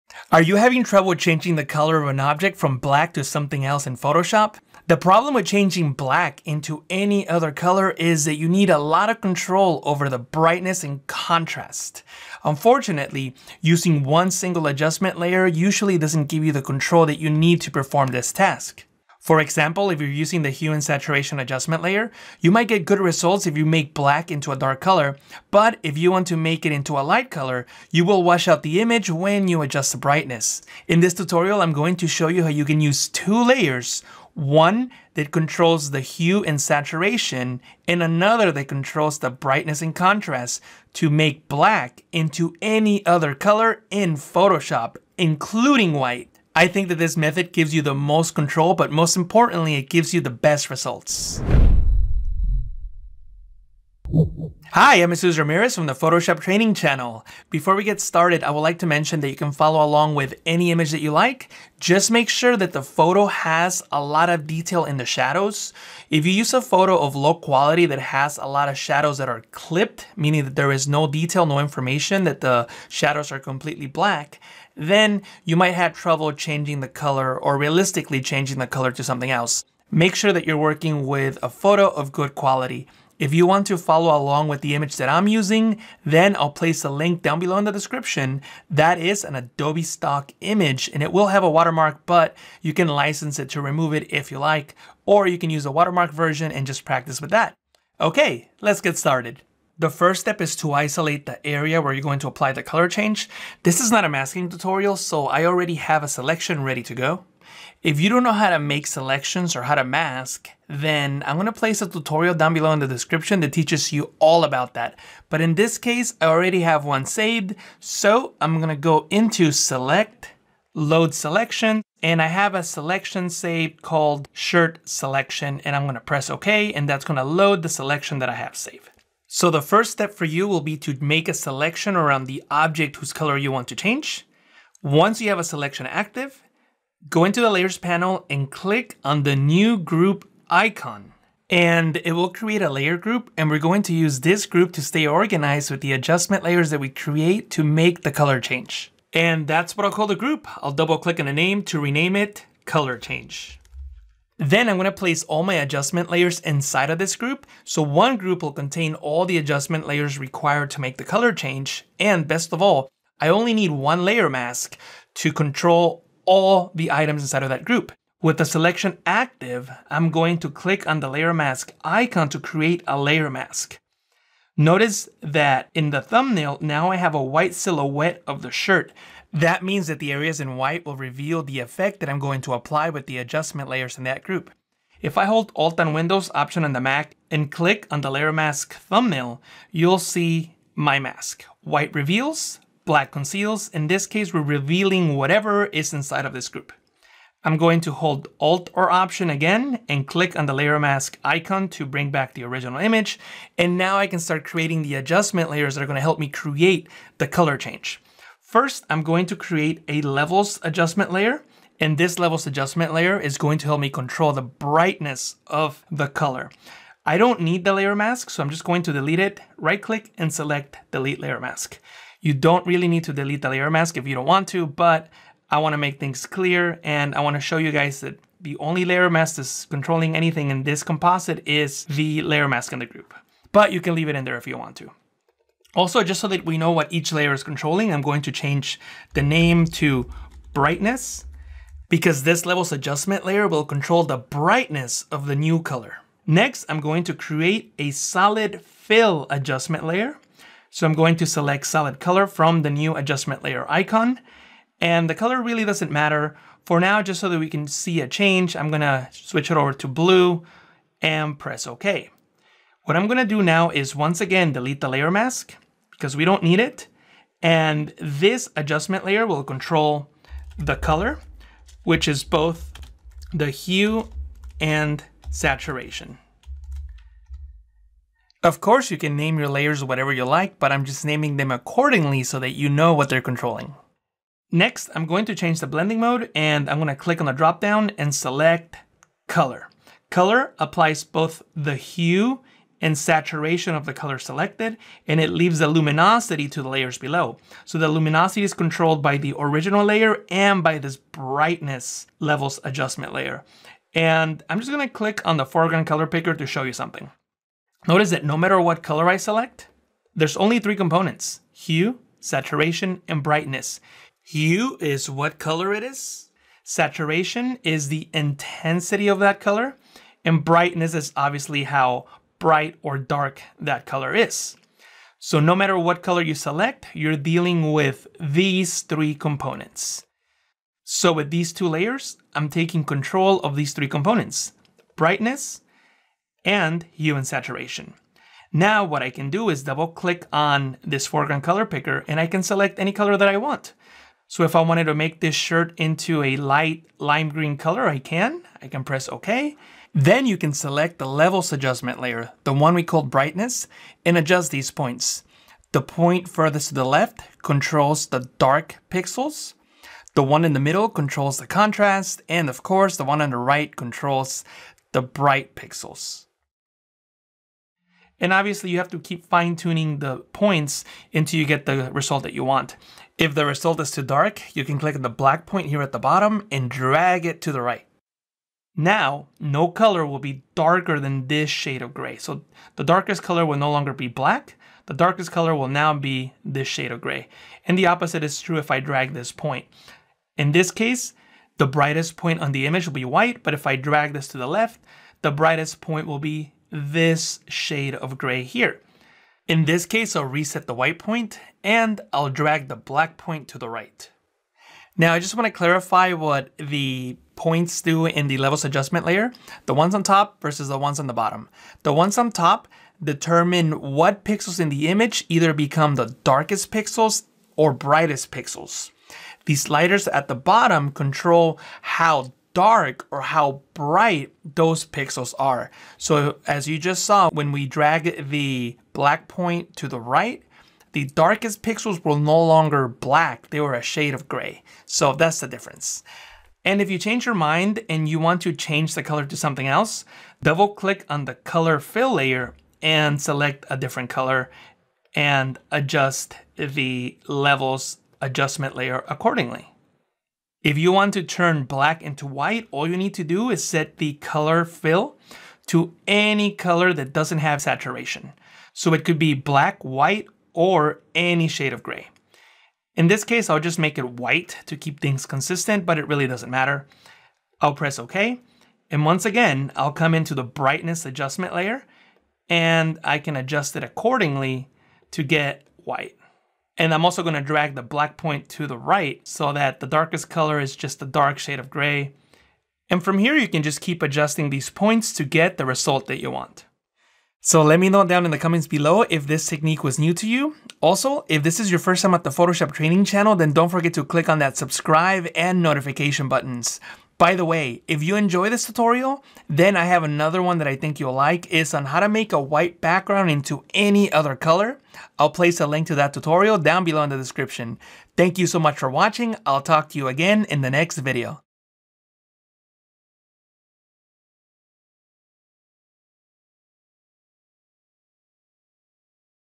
Best Way To Turn BLACK Into ANY COLOR in Photoshop [Including White!]
Learn a unique, easy-to-use, and powerful method to black into any color that you want (including white!) in Photoshop.

The challenge with changing black into any other colors is that you need a lot of control over the brightness and contrast.

Most techniques use a single Hue and Saturation Adjustment Layer to create the color swap, which can get you good results with dark colors. But if you want to make black into a light color,
then you will wash out the image when you adjust the brightness.

In this tutorial, I am going to show you how you can use
two layers, one that controls Hue and saturation, and another that controls brightness and contrast to turn black into any color in Photoshop, including white!

I think that this method gives you the most control,
and most importantly the best results!

📘 INDEX - Turn Black Into Any Color In Photoshop